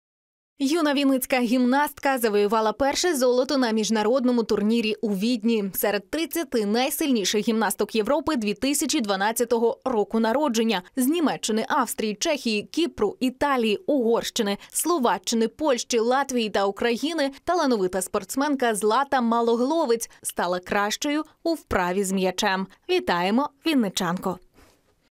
Вінницька гімнастка завоювала золото на міжнародному турнірі у Відні
Юна вінницька гімнастка завоювала золото на міжнародному турнірі у Відні. Серед 30 найсильніших гімнасток Європи 2012 р.н. талановита спортсменка Злата Малогловець стала кращою у вправі з м'ячем. Вітаємо вінничанку!